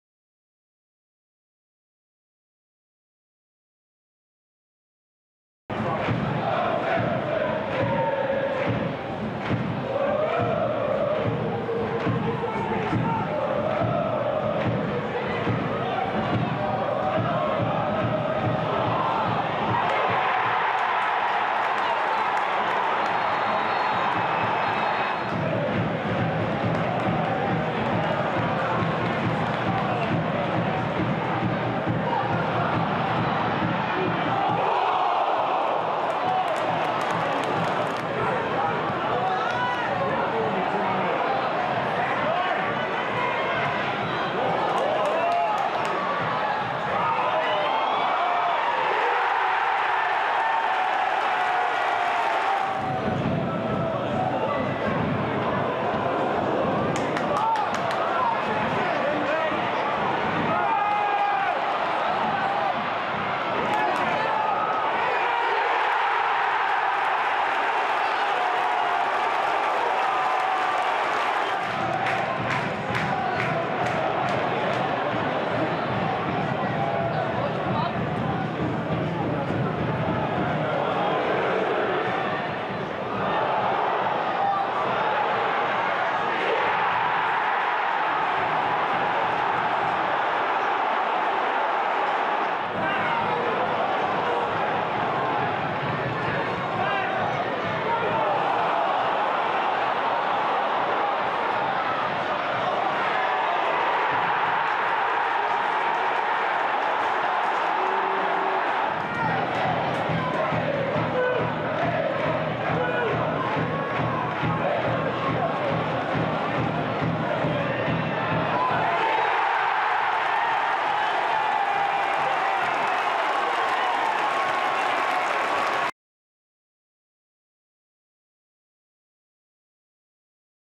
HIGHLIGHTS | NOTTS COUNTY 3-3 MK DONS
Sam Austin, Alassana Jatta and Aaron Nemane were on the scoresheet in our dramatic Easter Monday draw.